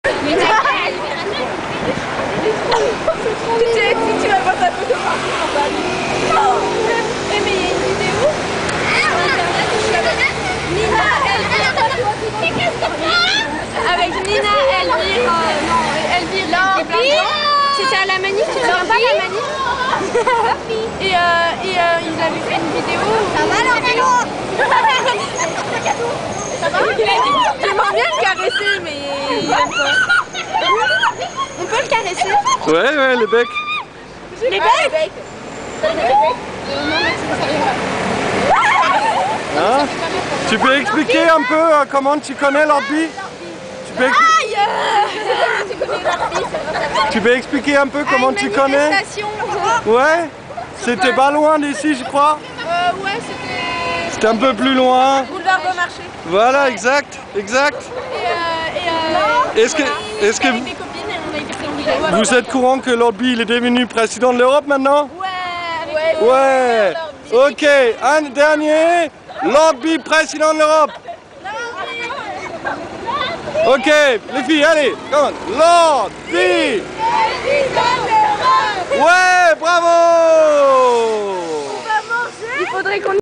Groupies avec Lord B
Un groupe de fans au place de la République joue chou chou avec The new Ecological President of Europe Lord B, Paris